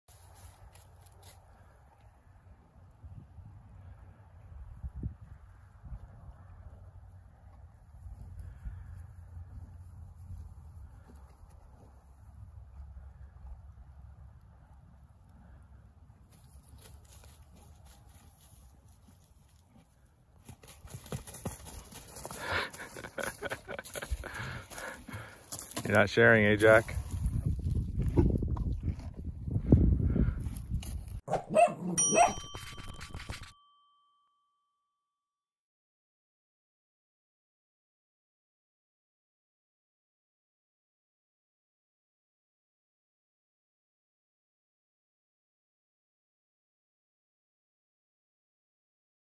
Greedy horse won’t share breakfast with hungry puppy
It looks like Jack is so hungry that he won't share a bite of his breakfast with Indy, the 7m Turkish Kangal puppy. Kids, let this be a lesson to all of you that sharing is caring, and Jack is not a good horsey!